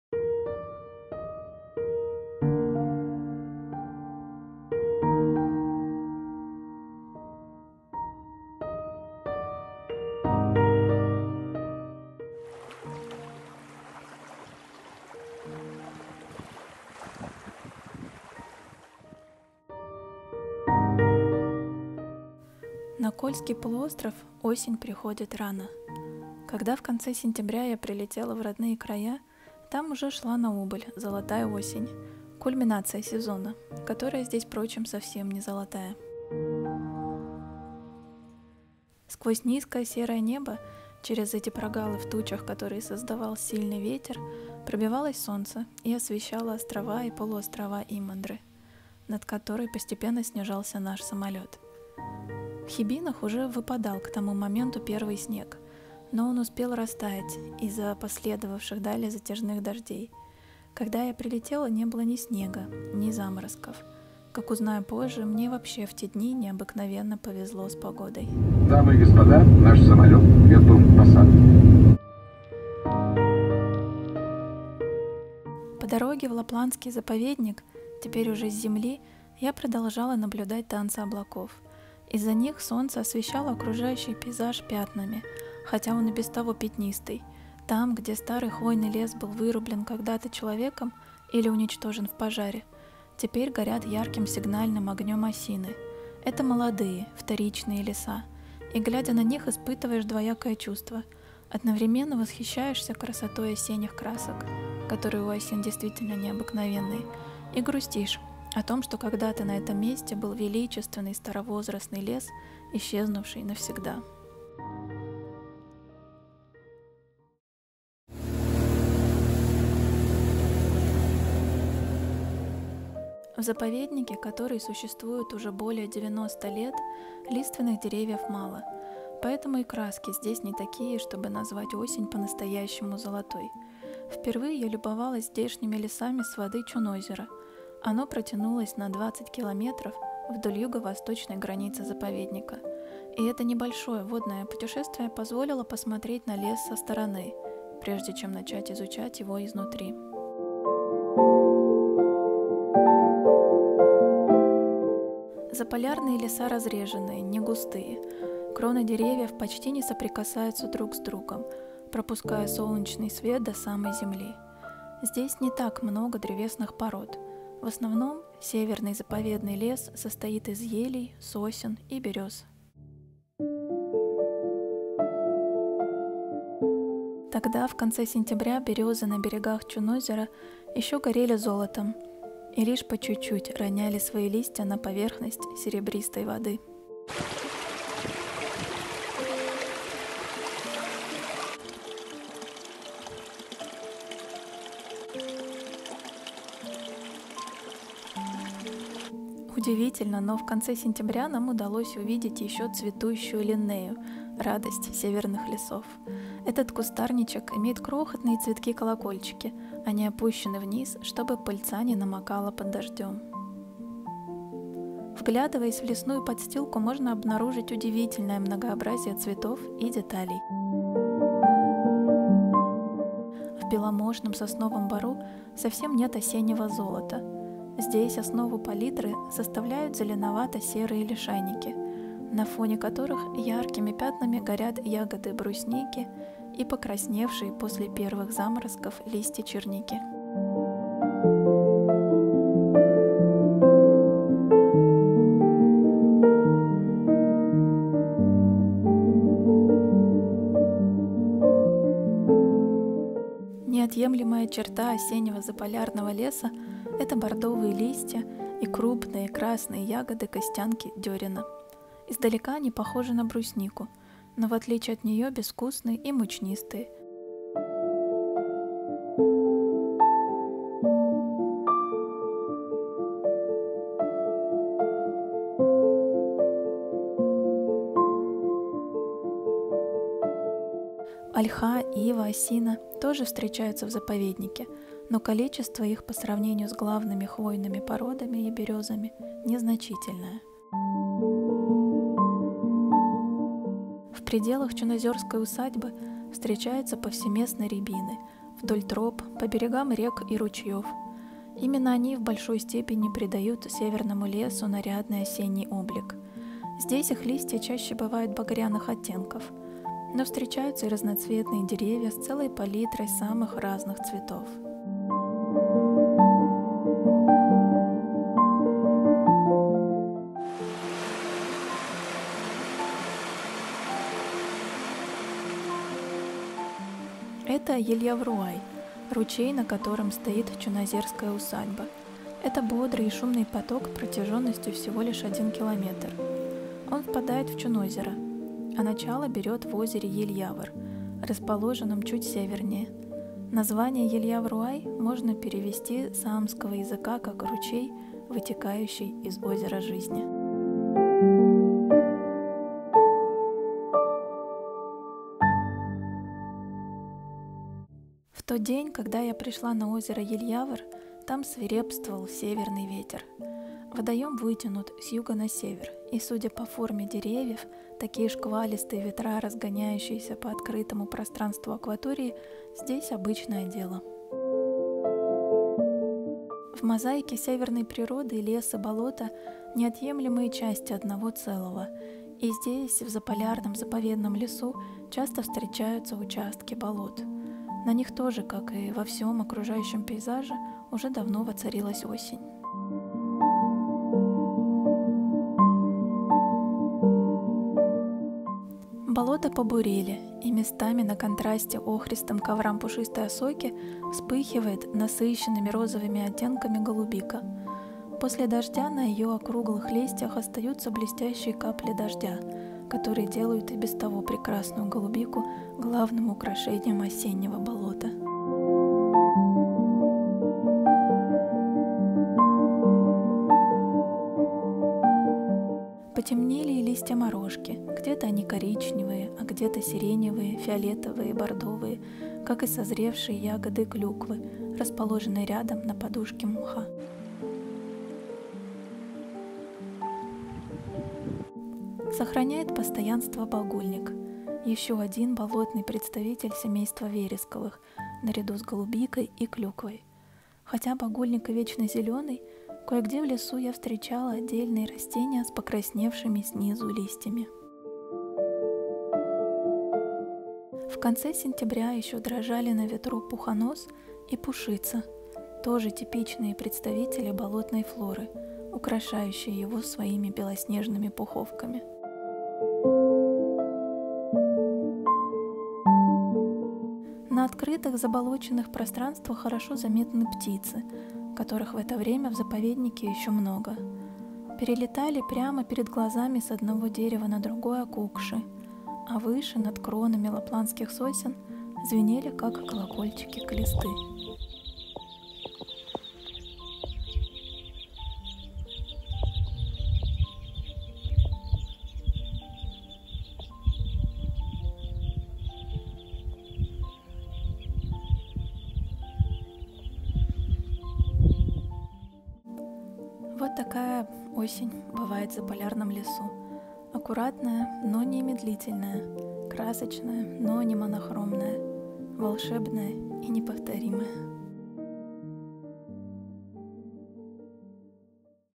осень в Лапландии — прогулка по заполярным лесам и болотам
Всем привет! В конце сентября в рамках рабочей поездки я посетила Лапландский заповедник и родную Кольскую землю. Я помогаю в работе студии документального кино, которая начала снимать фильм про первозданные леса России. Первой точкой на карте стала Мурманская область.

Хочу поделиться кадрами, которые лично я сняла за четыре дня в заповеднике, иллюстрирующие красочную и волшебную заполярную осень. Мы прогуляемся по северной тайге, посмотрим на краски берёз и рябин, увидим ручей, вытекающий из озера жизни, вдохнем просторы побуревших болот, пройдём на катере по Чунозеру и проводим взглядом стаю клестов над головой.